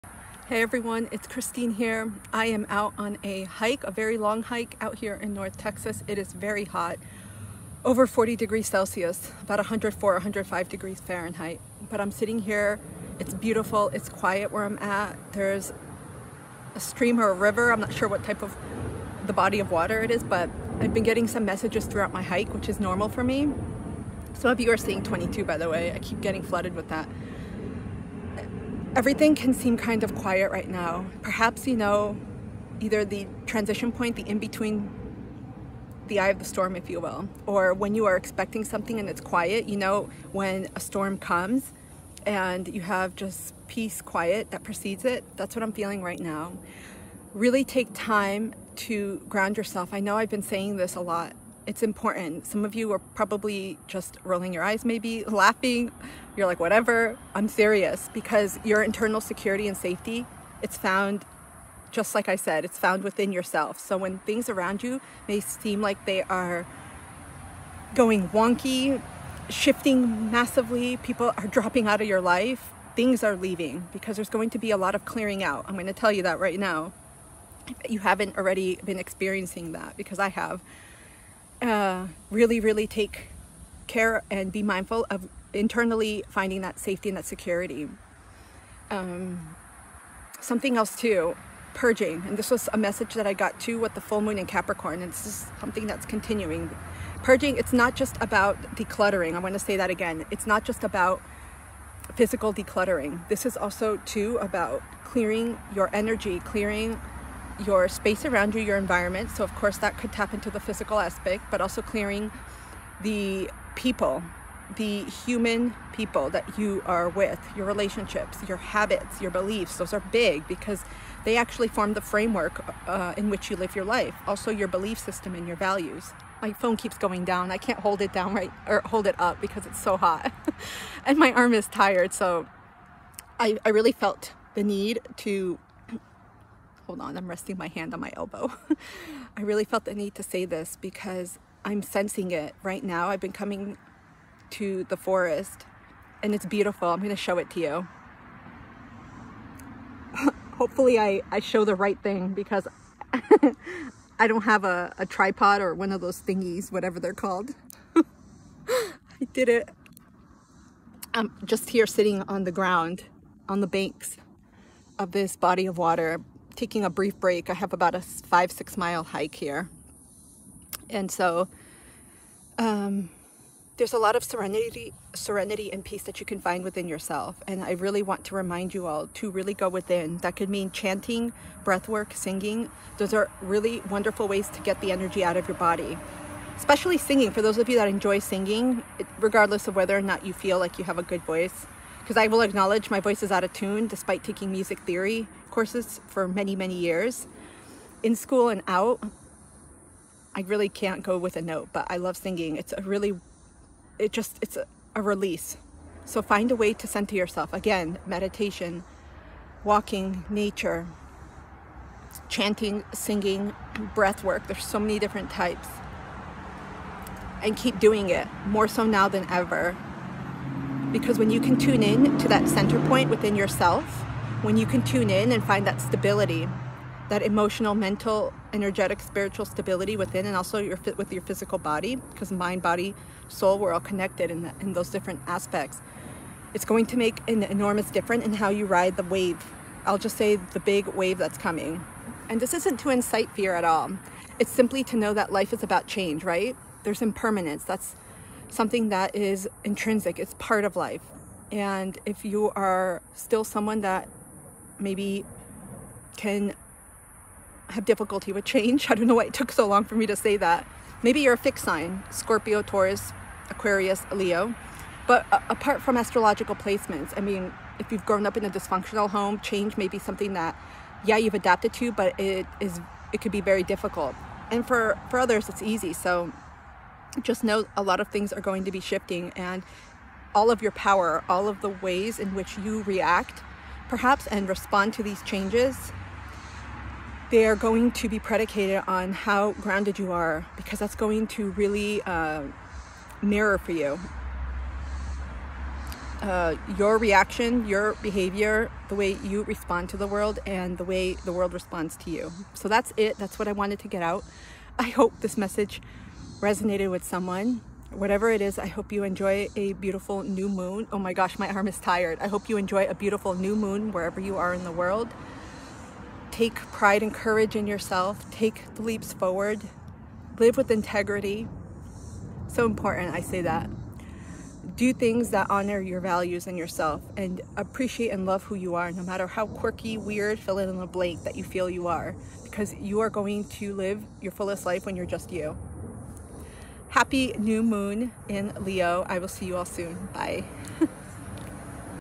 Channeled Message: Your Peace & Stability Are Closer Than You Think!
July 28, 2022. I tend to channel messages throughout my day, especially when I am hiking. I received a lot and wanted to share this with you as we are undergoing a very active and energetic week with the New Moon in Leo, the upcoming Uranus-Mars-North Node (in Taurus) conjunction, and the opening of the Lion's Gate 8/8 Portal. I added the date here for reference, but know that whenever this message finds you, there was something in there for you :)